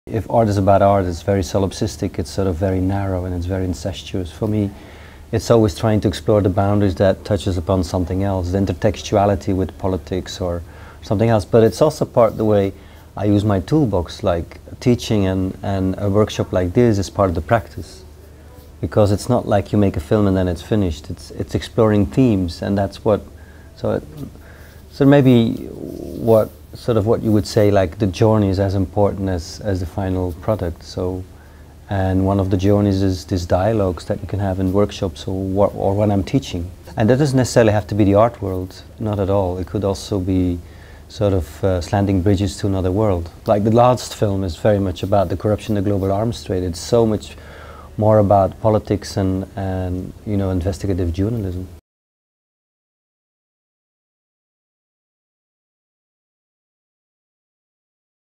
Komisariotza programa | Programa de comisarido: Johan Grimonprez
Prozesuaren garrantzia, beste esparruekin harremanak aztertzea, bere sorkuntzaren barne urrats garrantzitsua da Johan Grimonprez artistaren hitzetan.

La importancia del proceso, de explorar las conexiones con otros ámbitos es parte de la práctica del artista Johan Grimonprez.
tabakalera.eu/eu/komisariotza-ikerketa-eta-esparru-politikoa